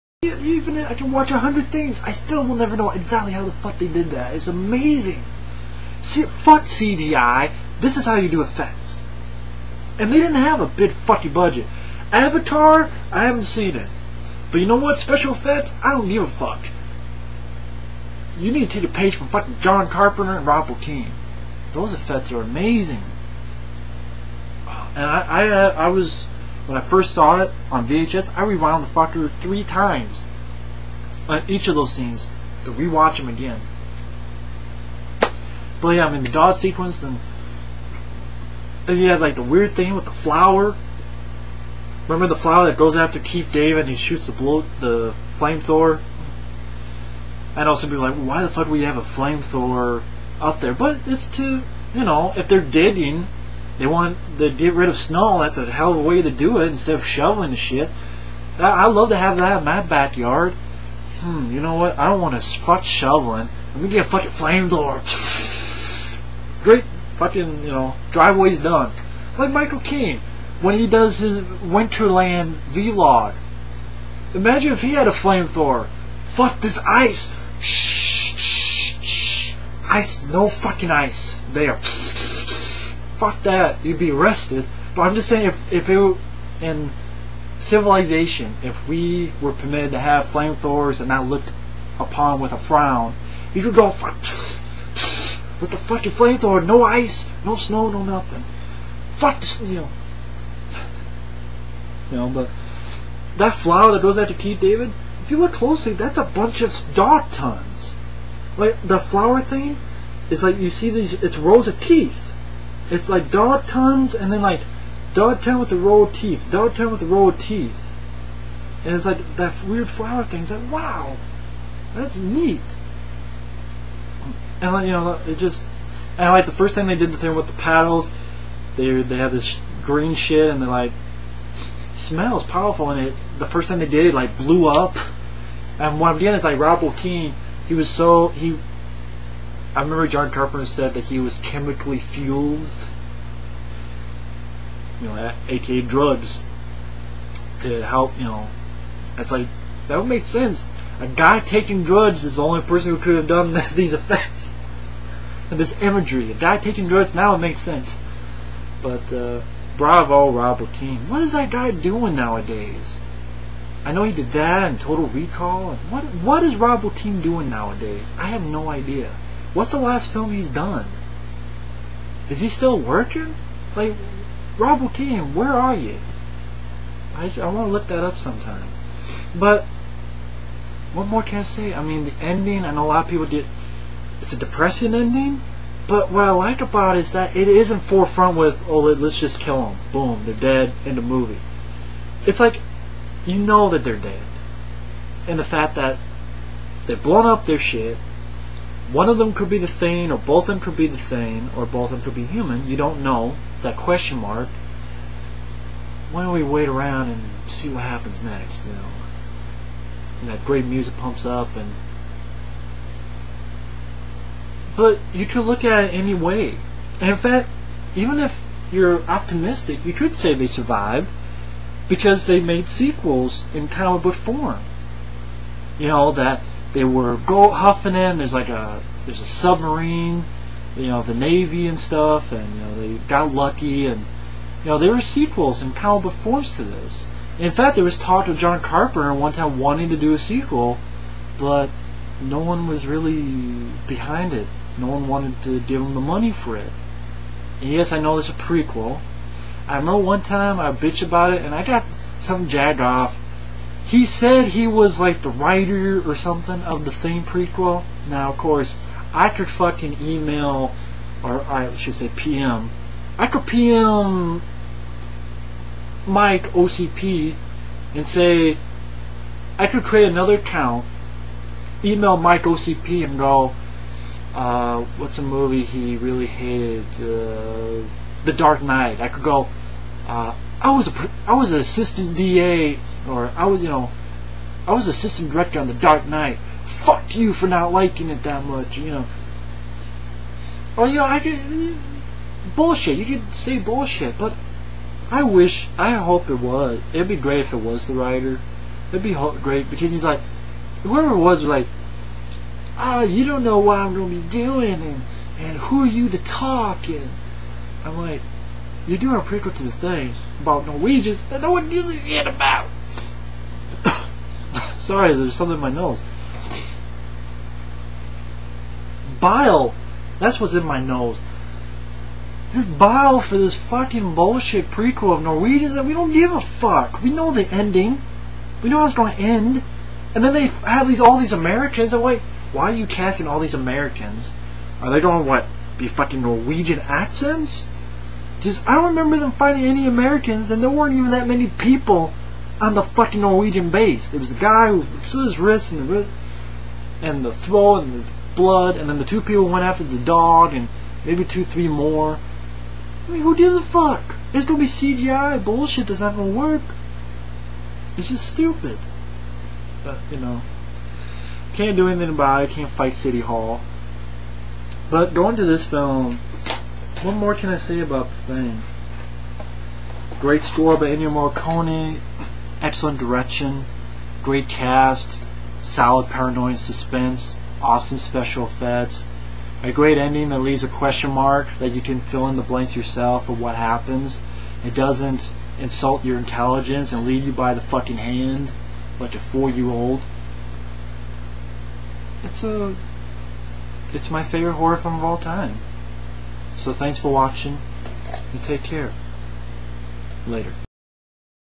John Carpenter's The Thing Movie Review Part 2 of 2
Last upload for the night. This time I talk about...without a doubt...my all time FAVORITE horror film..."John Carpenter's The Thing".

Carpenter, Kurt Russell, Keith David, Rob Bottin, & Ennio Morricone...class all the way. A+++ movie.